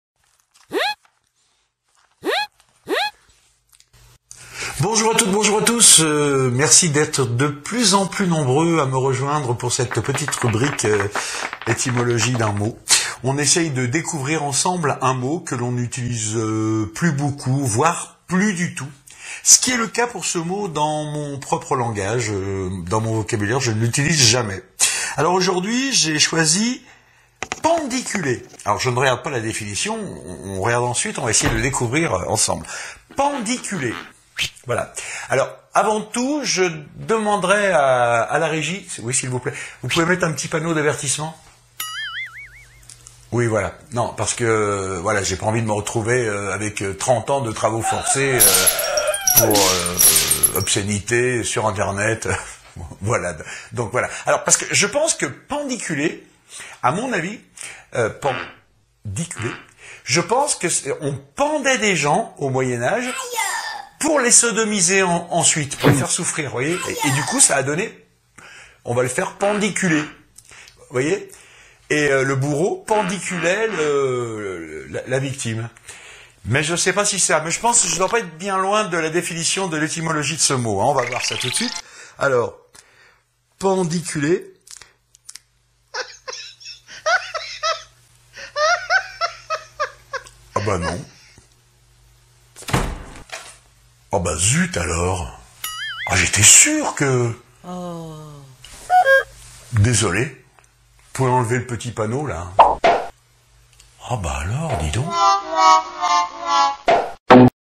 Etymologie d'un mot - Aujourd'hui "Pandiculer"
Régulièrement je vous explique un mot. Son origine, sons sens.
Enfin...J'essaye :-(

Merci d'avance  ;-)